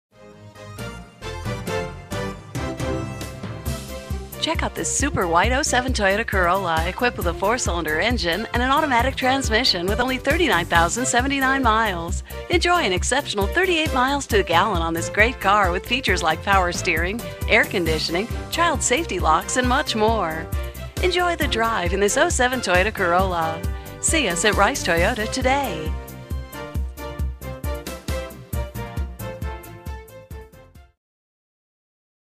2007 Toyota Corolla Rice Toyota Greensboro, NC 27408
2007 Toyota Corolla- check out this Super White 2007 Toyota Corolla, equipped with a 4 Cyl. engine and an automatic transmission with only 39,079 miles. enjoy an exceptional 38 miles to the gallon on this great car with features like Power Steering, Tachometer, Air Conditioning, Vanity Mirrors, Child Safety Locks, Anti-Theft Devices, Clock, and much more. Enjoy the drive and have peace of mind in this 2007 Toyota Corolla. see us at Rice Toyota today!
miles-  39,079
vin- 2T1BR32E47C769366

Rice Toyota
2630 Battleground
Greensboro, North Carolina 27408